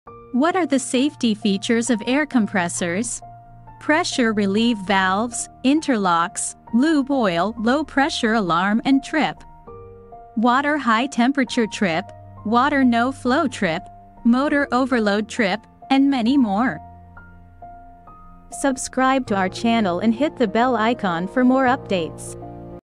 Safety features of Air compressors? || Minimac Systems
What are the safety features of Air compressors?

Pressure relieve valves, Interlocks, Lube Oil low-pressure alarm and trip, Water high-temperature trip, Water no-flow trip, Motor Overload trip, and many more.